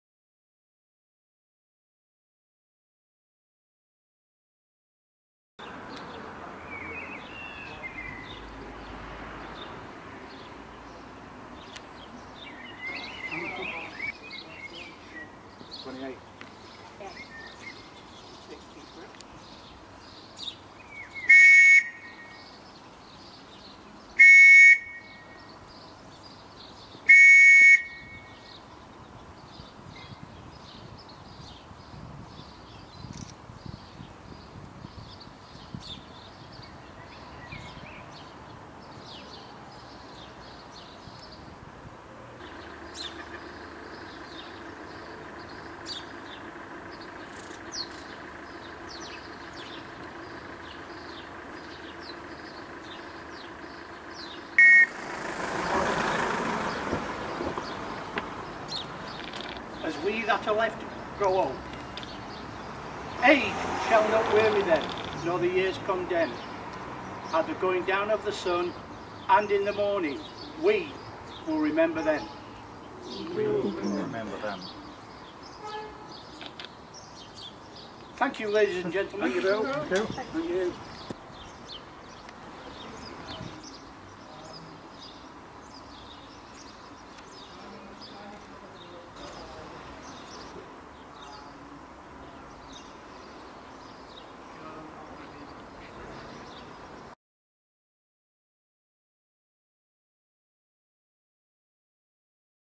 Commemoration of the start of the Battle of the Somme
In Saltash, Cornwall a few people gathered to  mark of the beginning of the Battle of the Somme 100 years ago